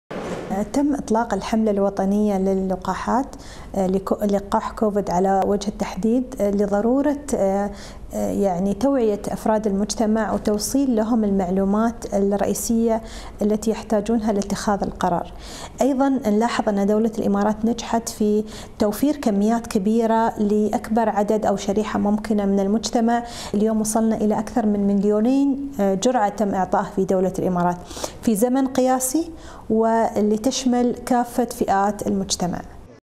لقاء د.فريدة الحوسني على قناة الحرة للتحدث عن لقاح كوفيد-19
لقاء د.فريدة الحوسني، مدير إدارة الأمراض السارية فـي مركز أبوظبي للصحة العامة، على قناة الحرة للتحدث عن لقاح كوفيد-19